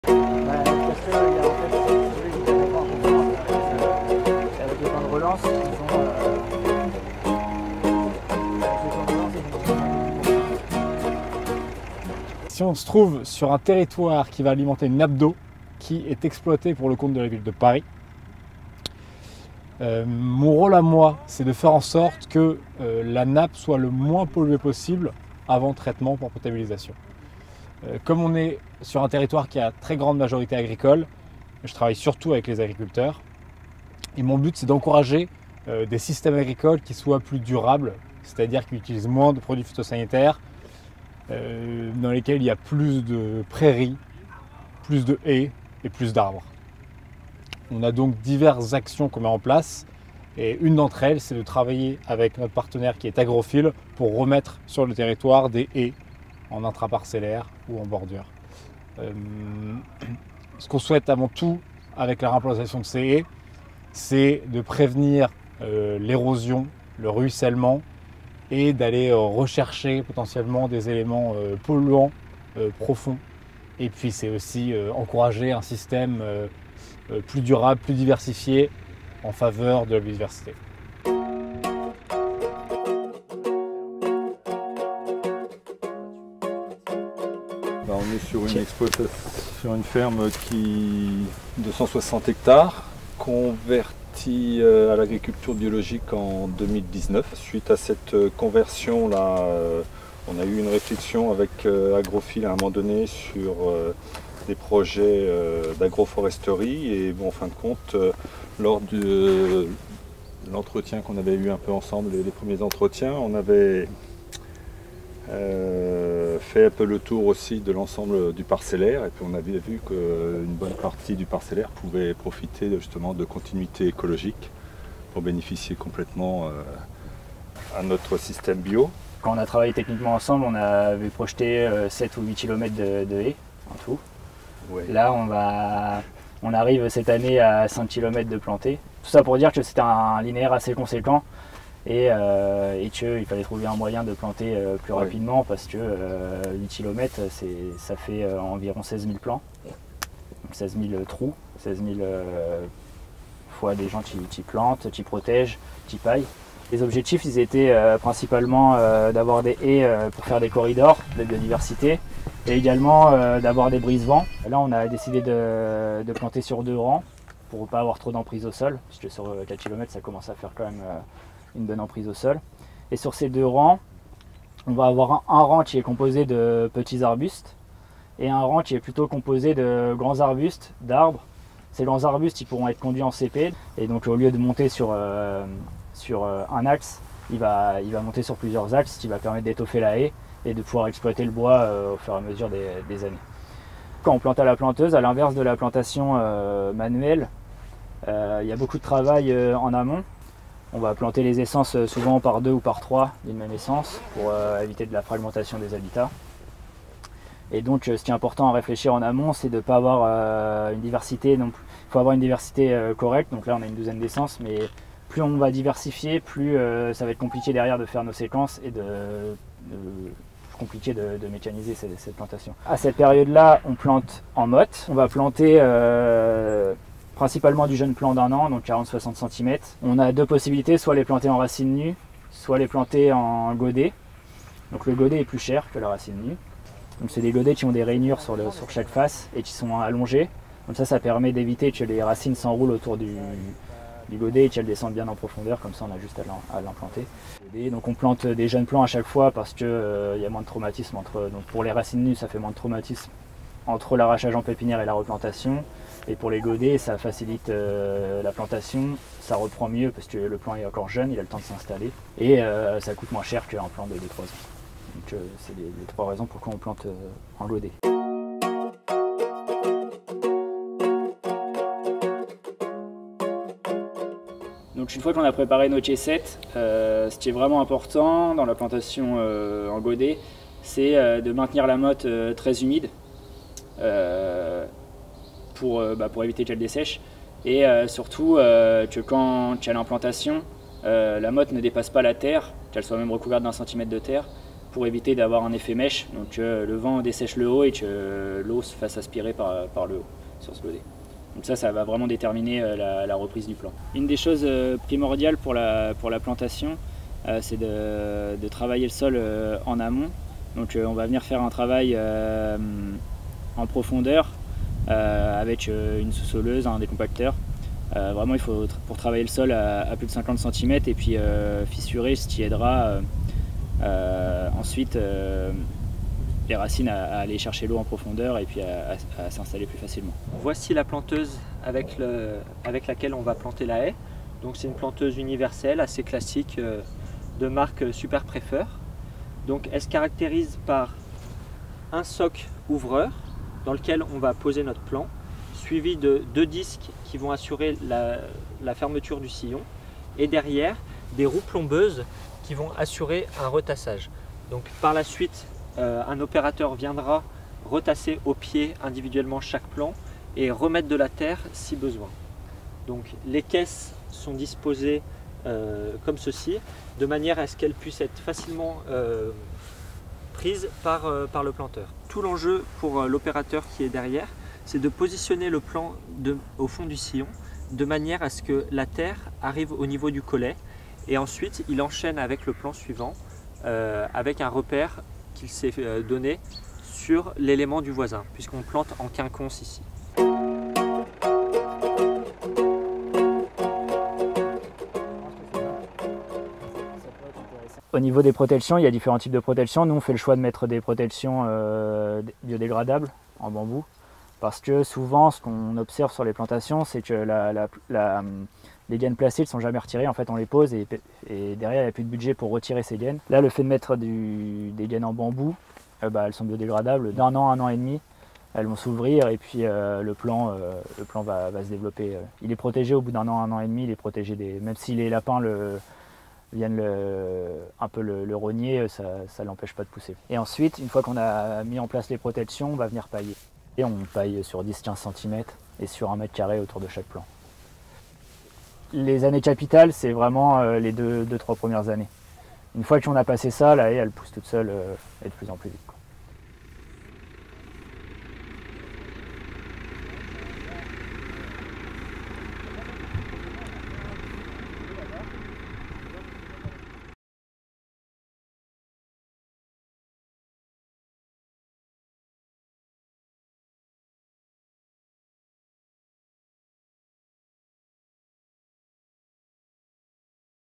Plantation mécanisée de haies (planteuse)
La plantation mécanisée d'arbres et arbustes pour constituer une haie est une solution qui permet dans certaines conditions de météo et de sol de gagner du temps sur la plantation.
Le 10 novembre 2022, Laurent Vignier, EARL de Rouillot, accueillait ses voisins agriculteurs lors d'un chantier organisé par l'association Agrof'Île, pour montrer la plantation mécanisée de ses haies. Le projet a été soutenu par Eau de Paris, pour des services recherchés sur la préservation de la biodiversité, du climat et des ressources en eau.